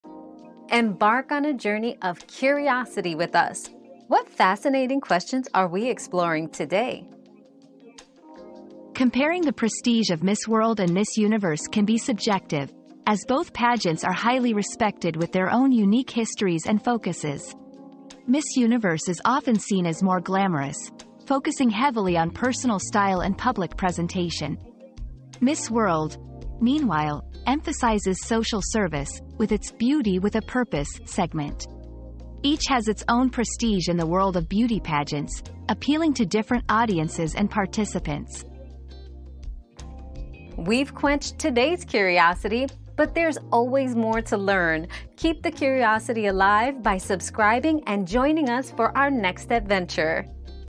Which is more prestigious Miss World or Miss Universe?
Miss World vs Miss Universe: Which is More Prestigious? 👉 Beauty Pageant Prestige 👉 Discover the differences between Miss World and Miss Universe, two highly respected beauty pageants with their own unique histories and focuses. Explore the glamour and personal style of Miss Universe, and the emphasis on social service in Miss World's 'Beauty with a Purpose' segment. Find out which pageant appeals to different audiences and participants, and decide which one you think holds more prestige.

Laura S. Harris (2024, February 11.) Which is more prestigious Miss World or Miss Universe?
    🌐 AskAbout.video